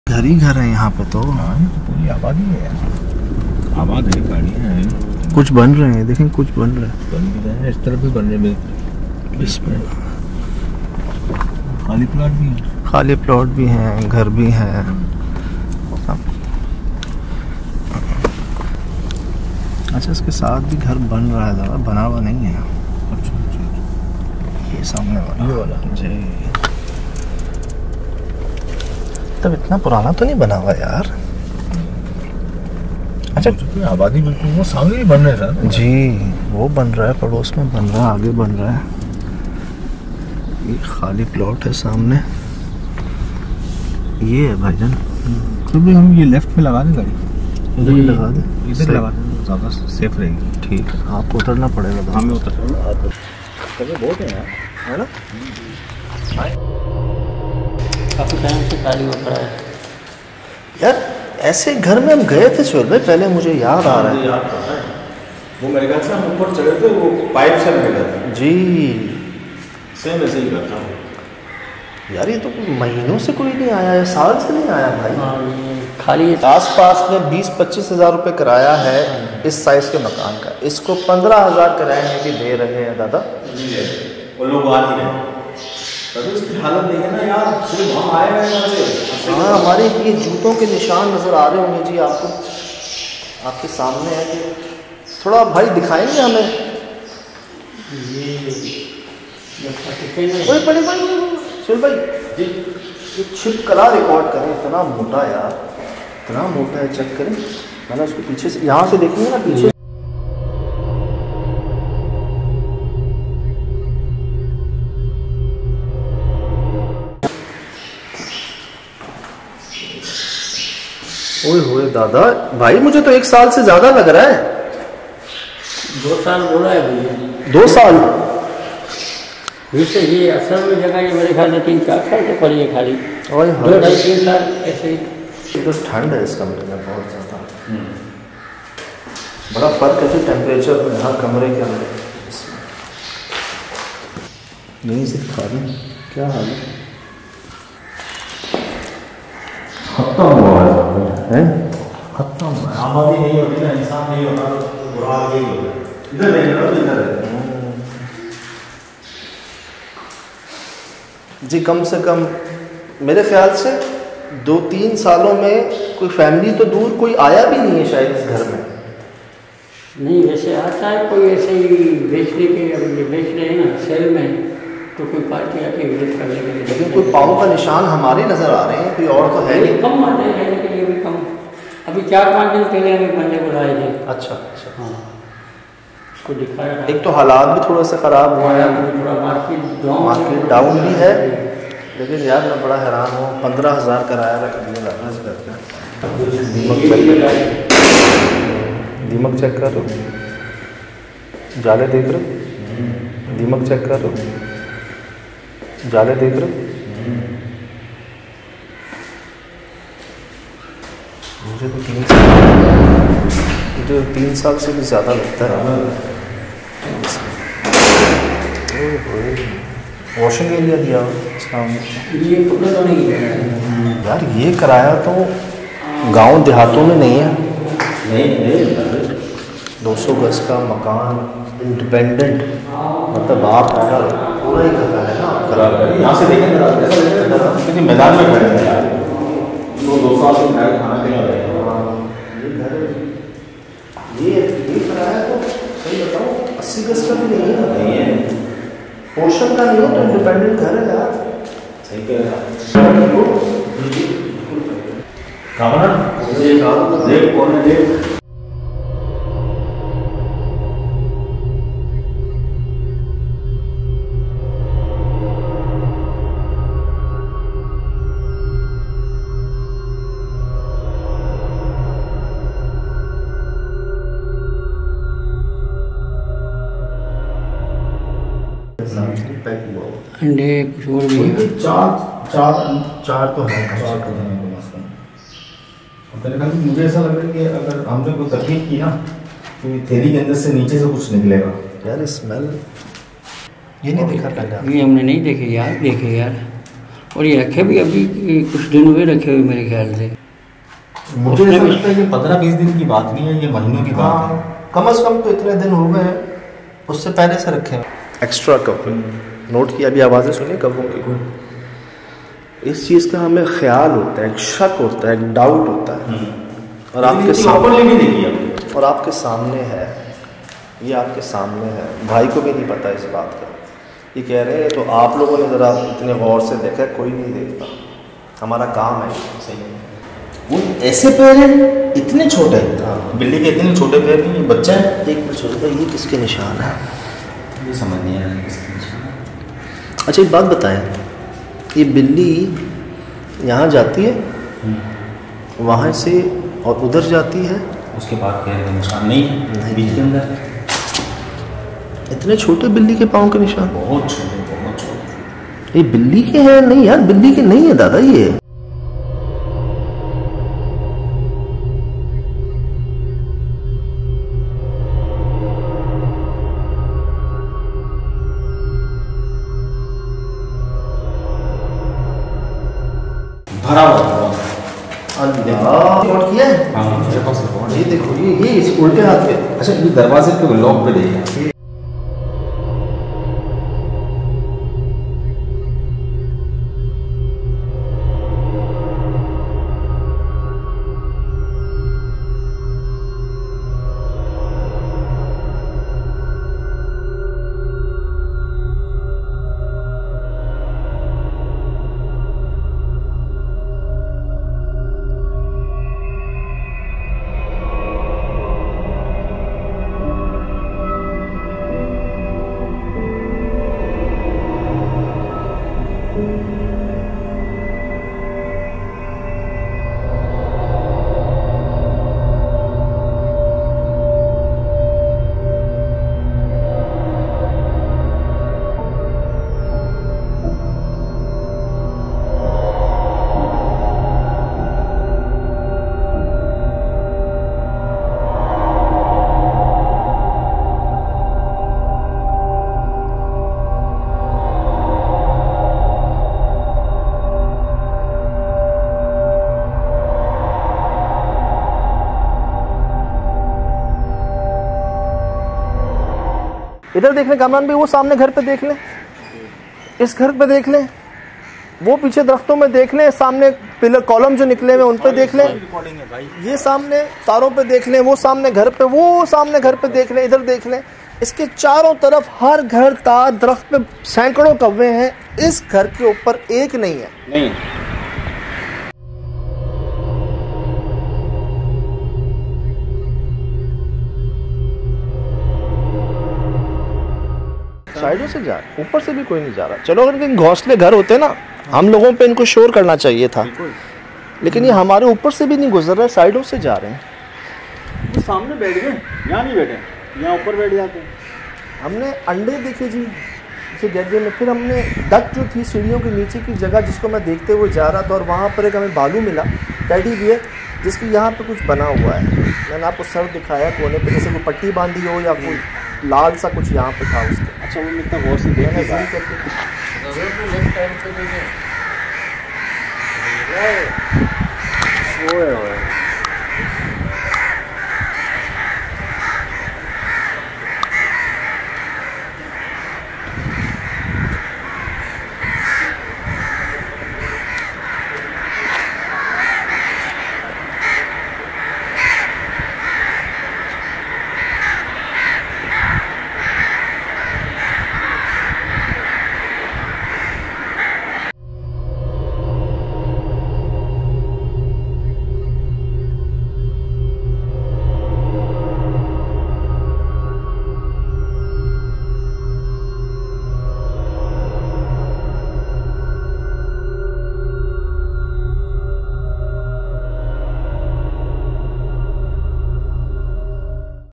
SSD 1413 | Is ghar main Jinnaat ko Jaadu k zariyay bheja gaya..?
jinn stories

Our Team:
Sajjad Saleem
Sohail Latif
Shiraz Irani
Sajid Baloch
Kamran Baloch